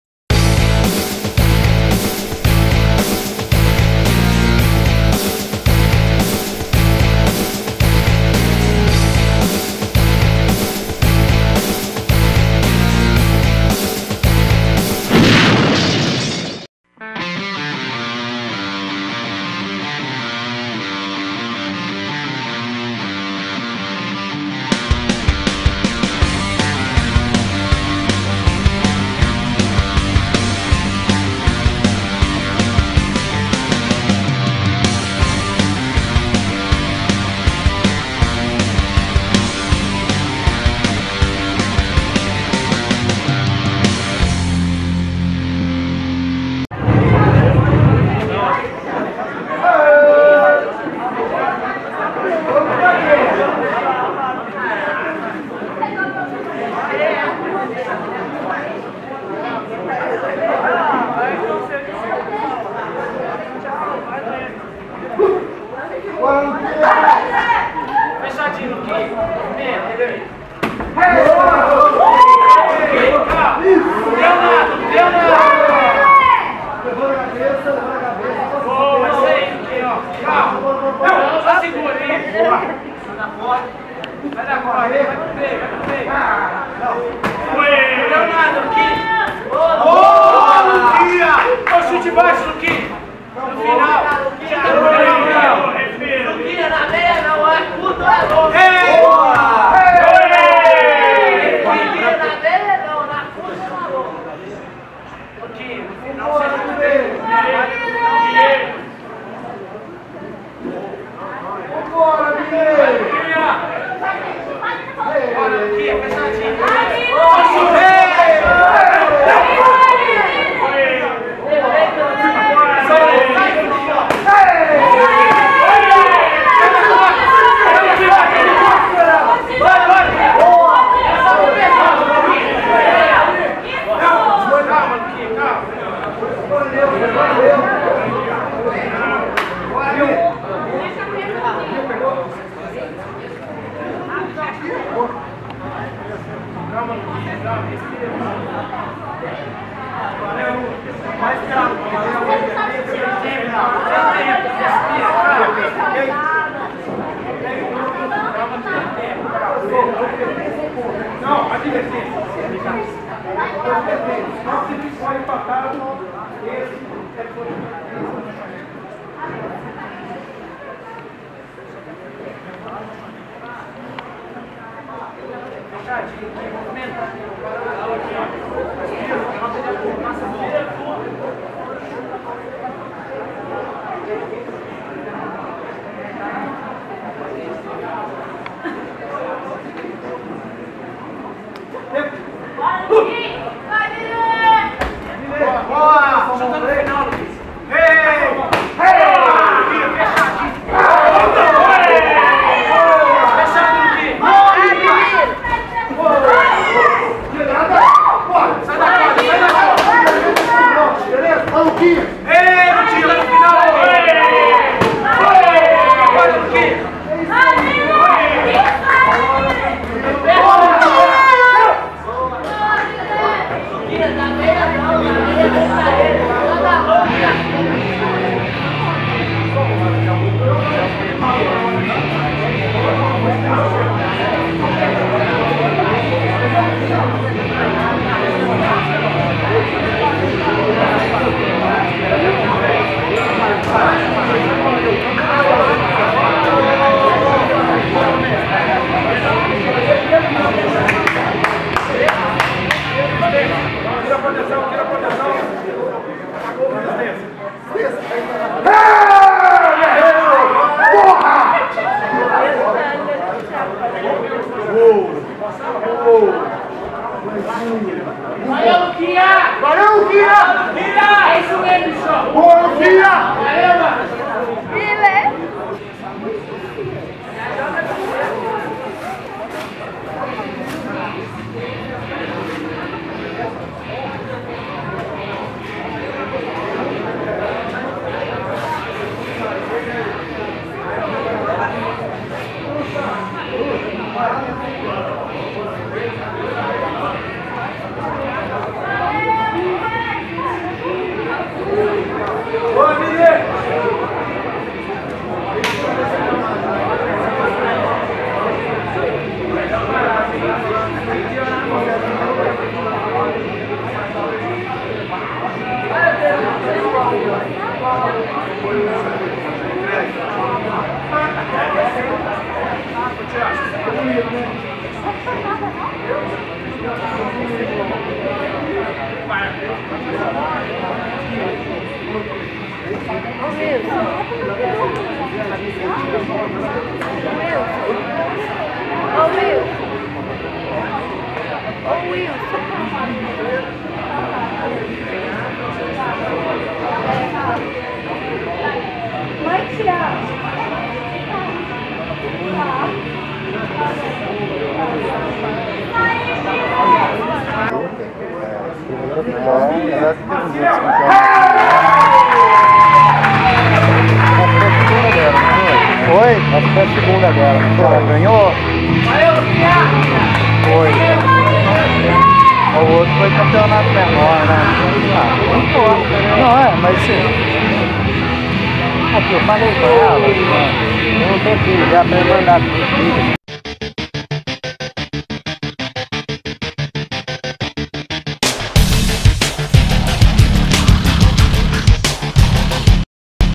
Lucas de Jesus vs Miller Maciel - Campeonato Mineiro 2014 CBKT
No co-main event da noite, Miller Maciel (Wilson Fight) venceu Lucas de Jesus (CT Luiz Muay Thai) por desistência na volta para o segundo round.